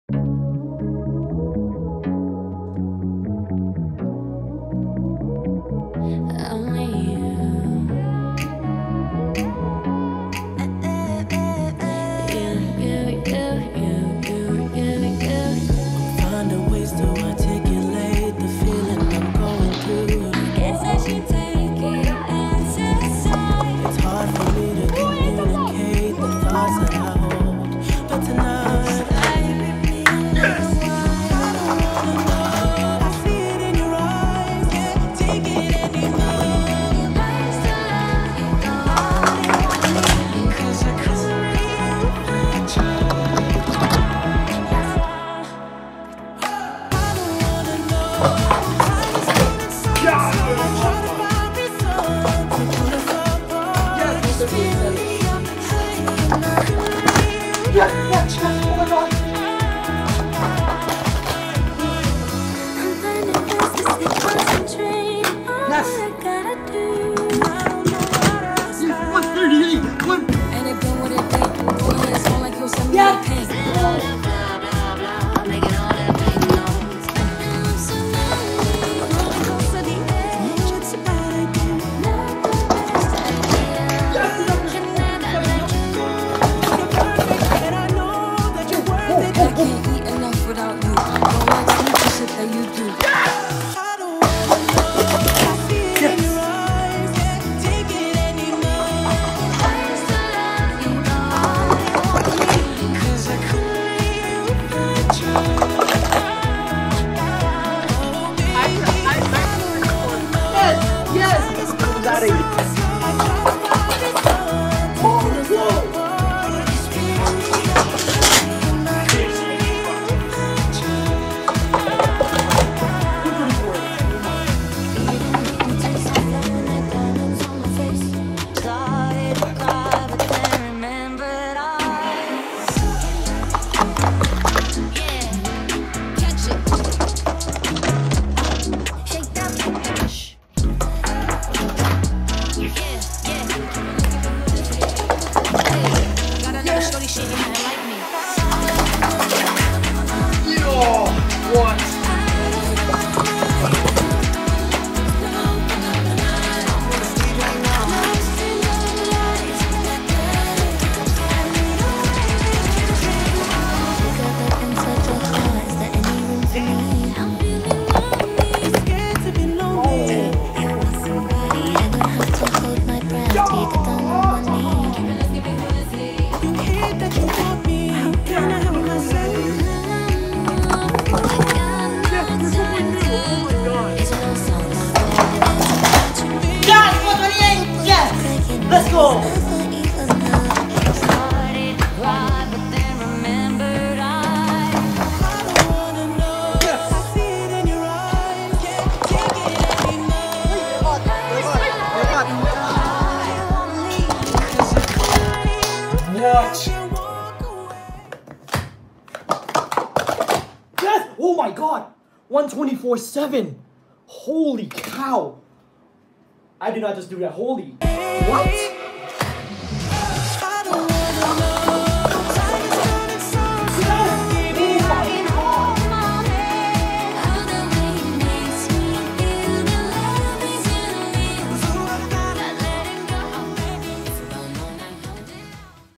Sport Stacking: Top 50 333 of 2023
And that officially wraps up the 'top ___' completely.

I made this video the best of all time over the years and it's safe to say, my work here is done. I'm happy to complete this and I'm putting this all to rest, on my end. Here's to the future 'Top ____' videos :)